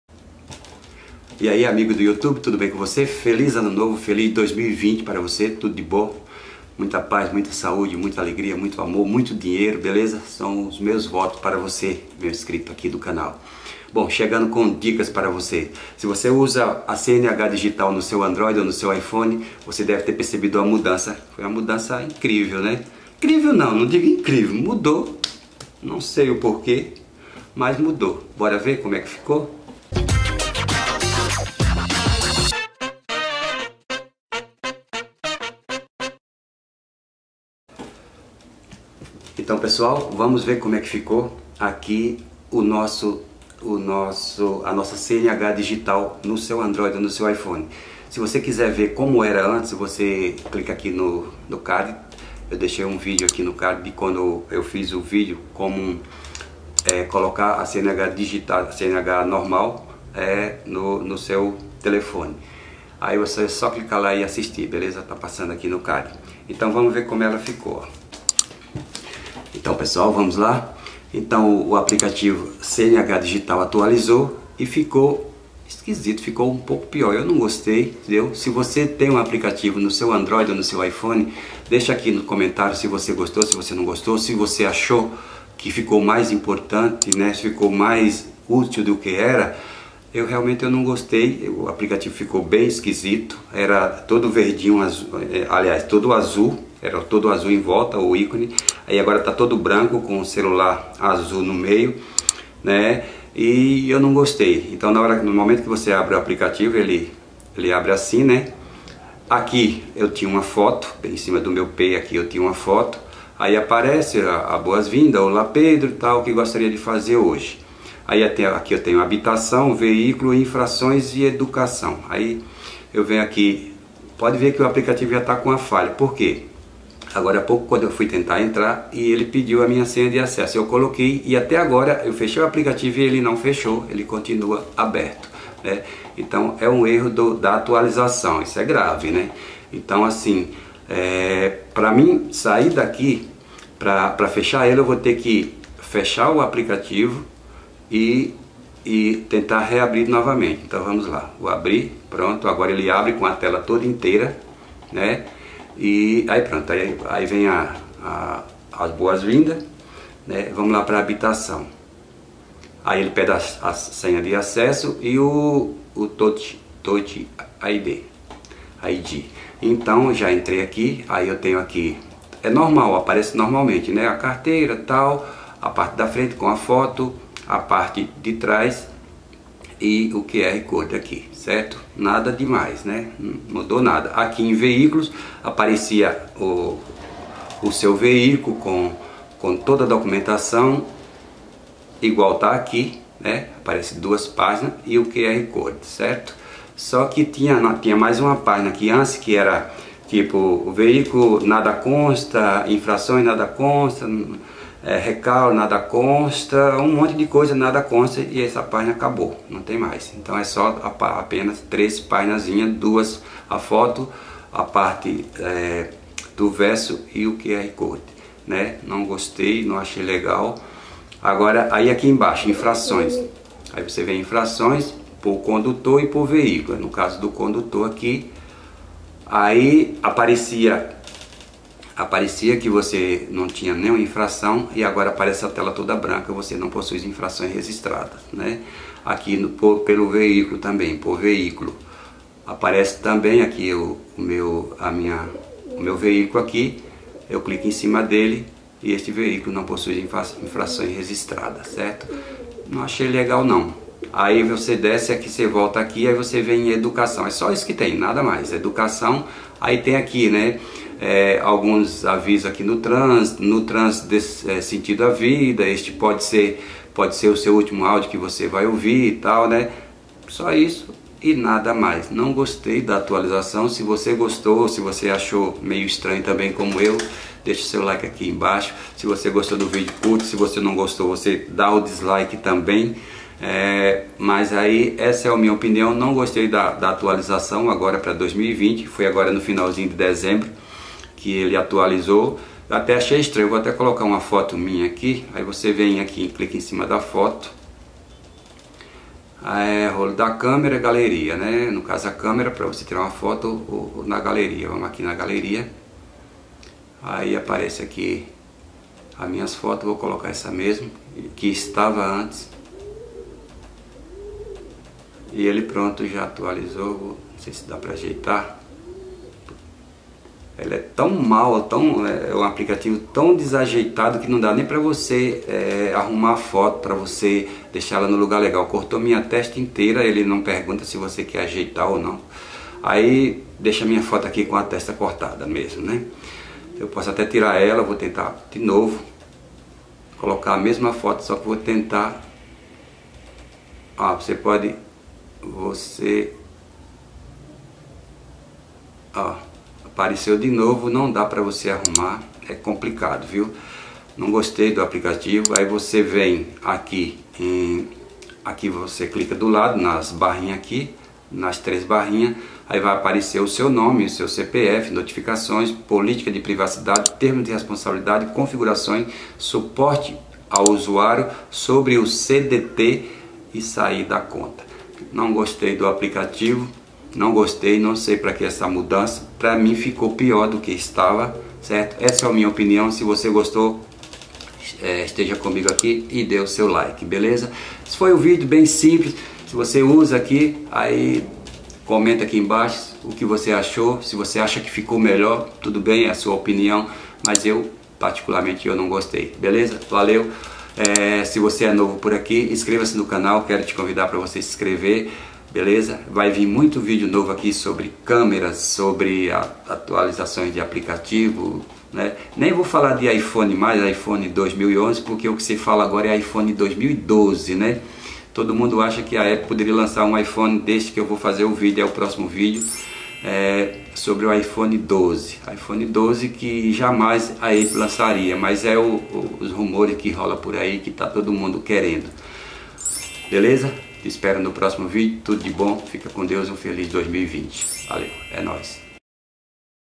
CARTEIRA CNH DIGITAL ATUALIZADA 2020
FAÇA UMA DOAÇÃO, PRECISO MUITO DE SUA AJUDA

AG: 4155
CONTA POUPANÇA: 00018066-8
OP:013
PEDRO SILVA OLIVEIRA

Nesse vídeo você vai saber e entender como ficou a CNH DIGITAL, na minha opinião não achei muito legal.

Muito obrigado por está no meu canal! Valeu! Grande abraço!